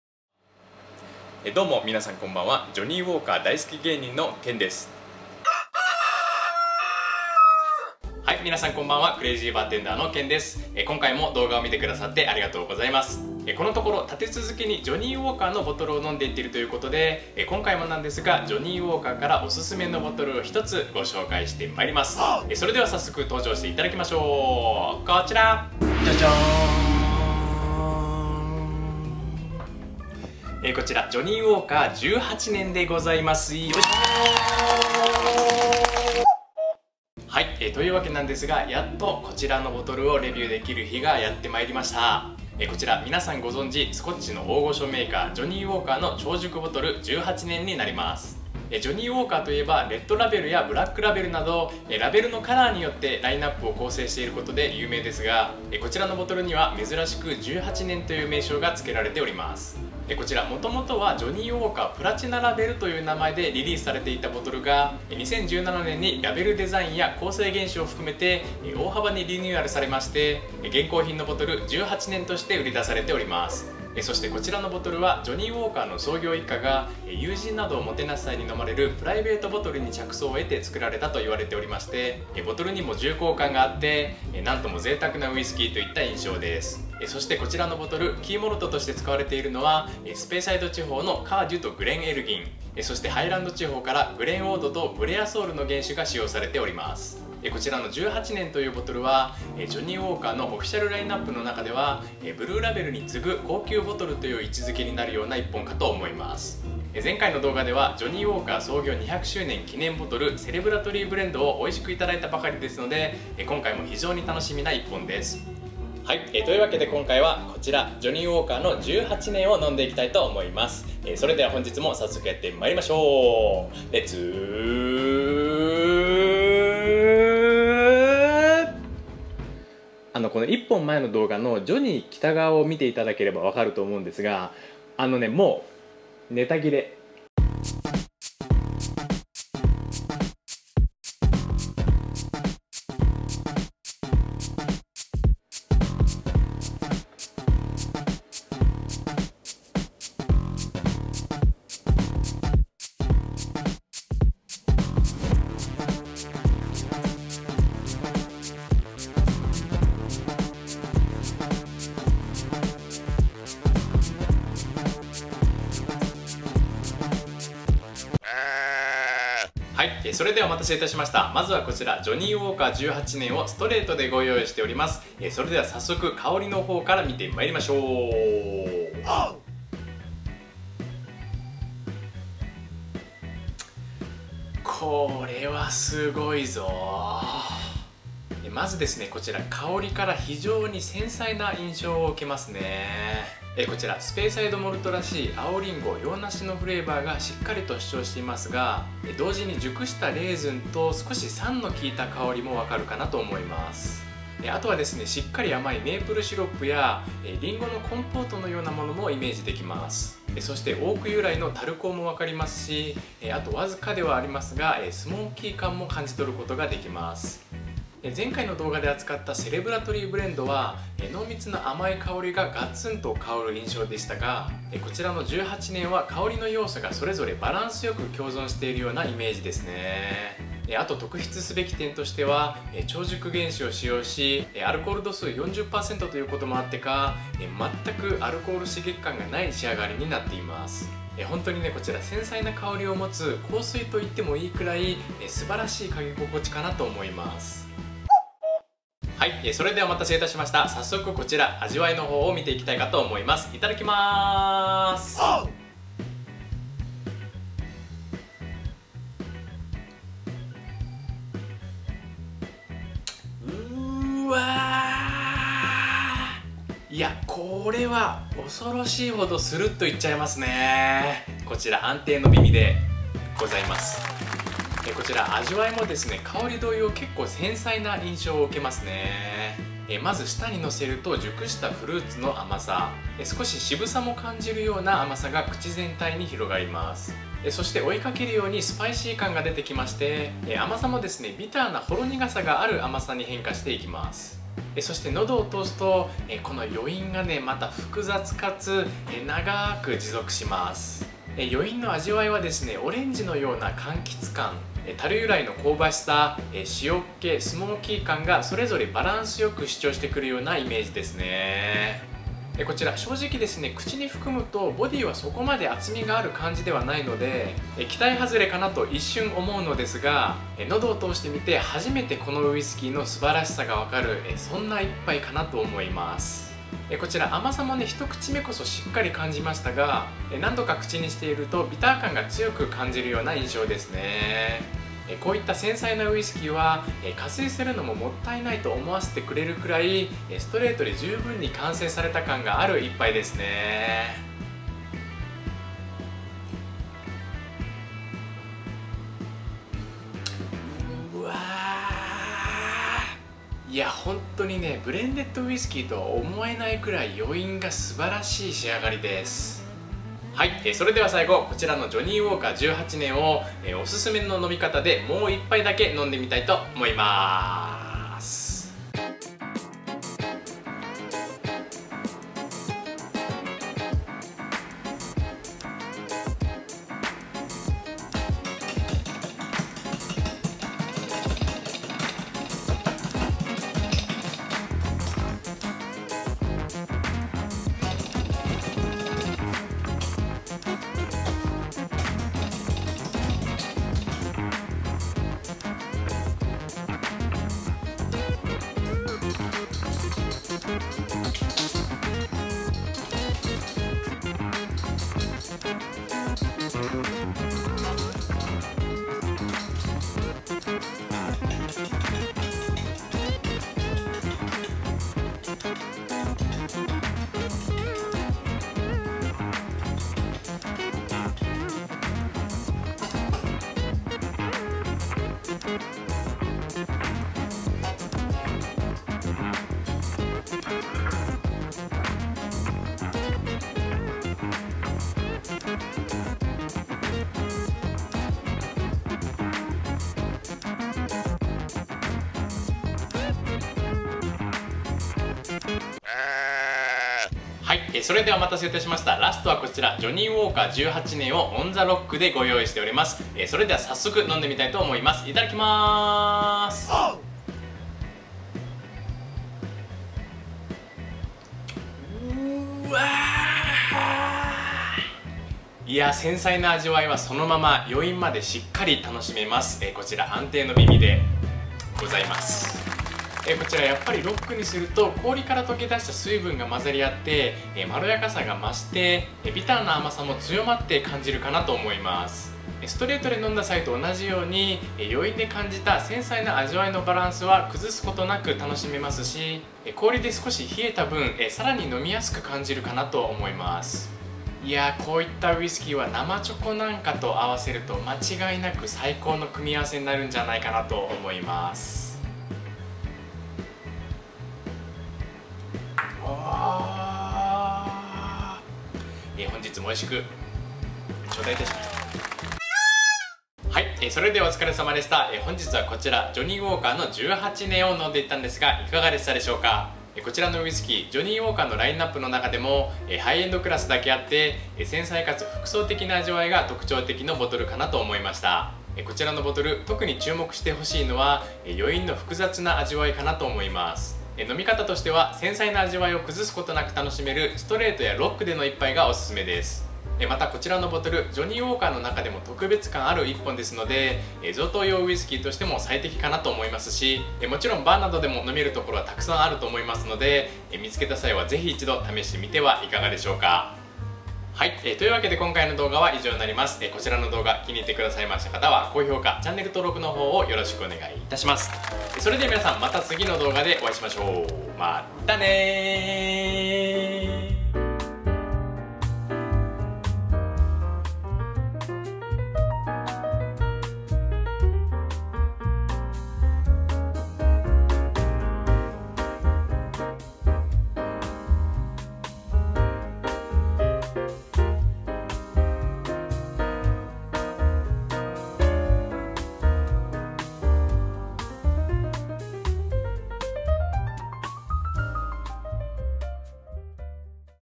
【贅沢品】プレゼントにも最適!!『ジョニー・ウォーカー18年』を現役バーテンダーが開封レビュー!!
皆さんこんばんわ🌙

今回はブレンデッドスコッチの名門ジョニー・ウォーカーより
【ジョニー・ウォーカー18年】を開封レビューして参ります✨

いくつもの味わいがバランス良く共存しているこちらのボトル
『繊細』という言葉がピッタリ当てはまるようなウイスキーかなと思います🥃

万人受けするような味わいかつ、ボトルデザインも美しく重厚ですのでプレゼントにもピッタリです👍

※お酒は20歳になってから、マナーを守って楽しく飲みましょう。